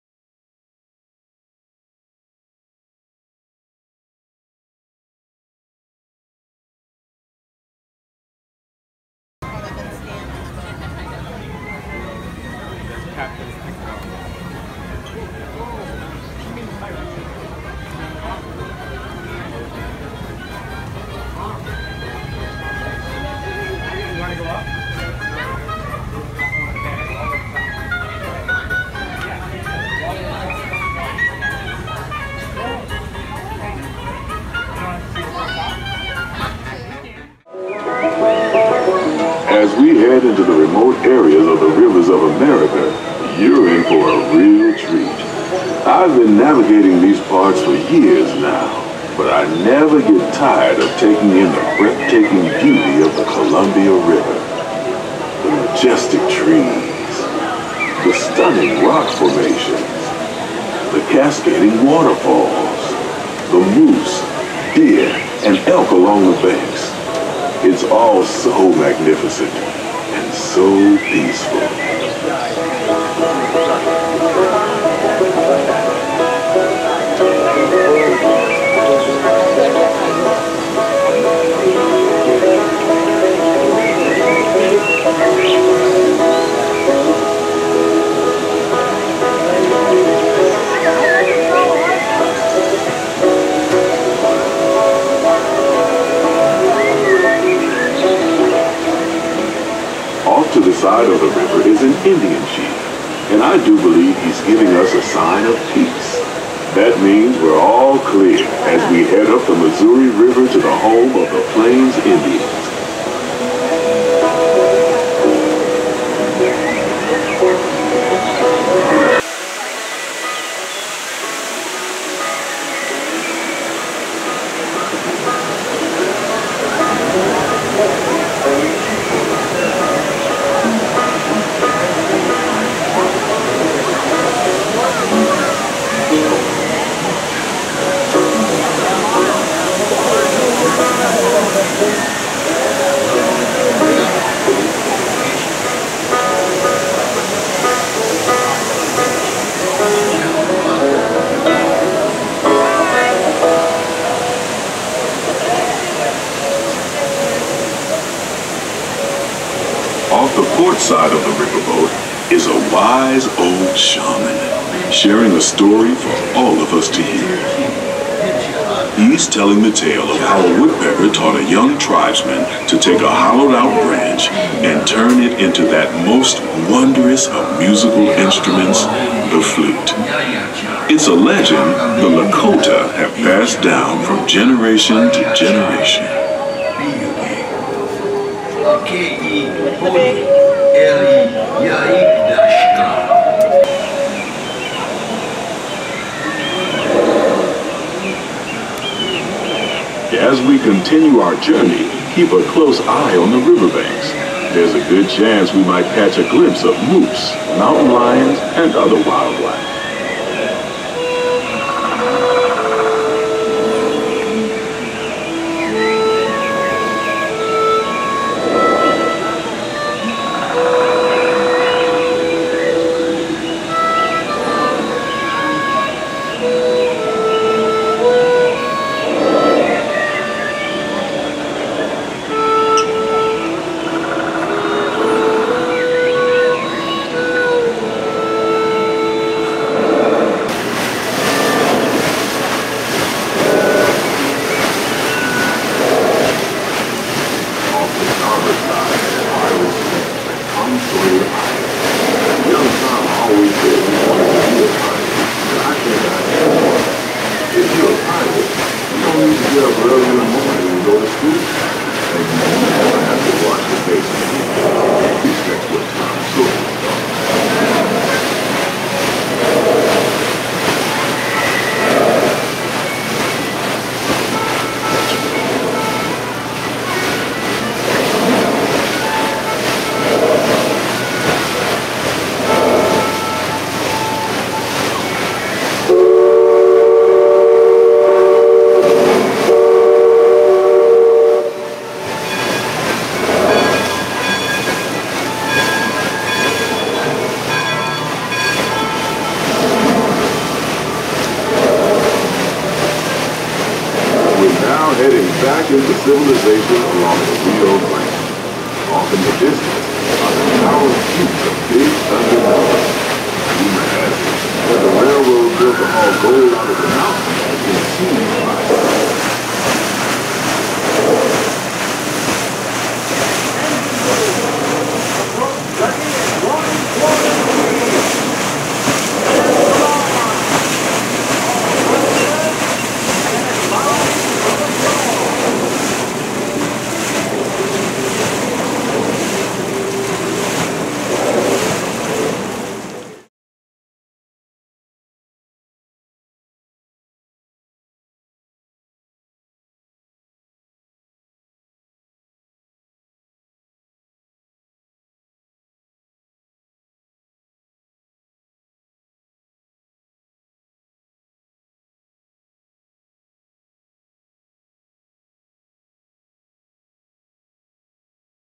Disneyland - 10/13/17 Star Wars: Galaxy's Edge view from Mark Twain Riverboat
Documenting the progress of Star Wars Land and the Rivers of America at Disneyland since March 2, 2016.

Lots of fun links!

4/15/17: The creative minds from Lucasfilm and Walt Disney Imagineering discuss their unique partnership and share details about the new galactic Star Wars-themed lands being created at Disneyland and Walt Disney World Resorts. To read more, visit the Disney Parks Blog

Go to 9:48 on video for SWL aerial views